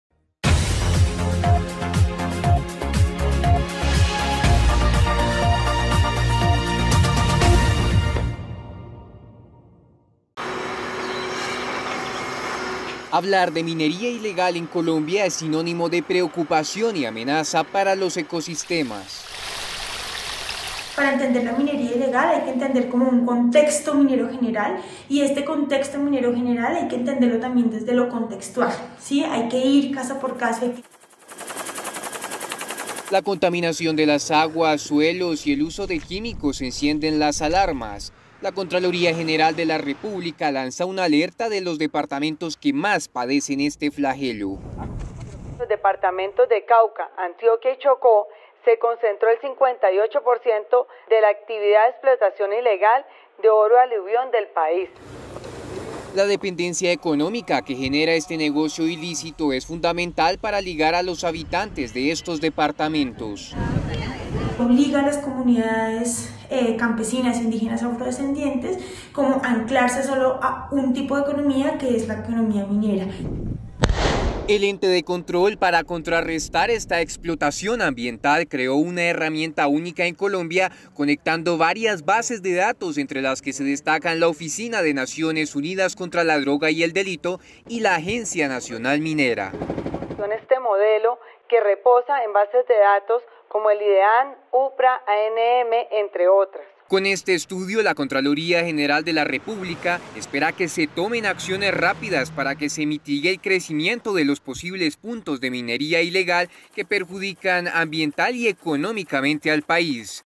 Alerta por el crecimiento de minería ilegal
Un flagelo que crece cada día en Colombia y unas de las principales rentas de los actores al margen de la ley sigue aumentando en el país. La Contraloría General de la República reveló cuales son las zonas donde más está creciendo el impacto de la minería ilegal hoy en Colombia. En nuestro espacio ambiental con la cortesía de noticias RPTV, tendremos el informe.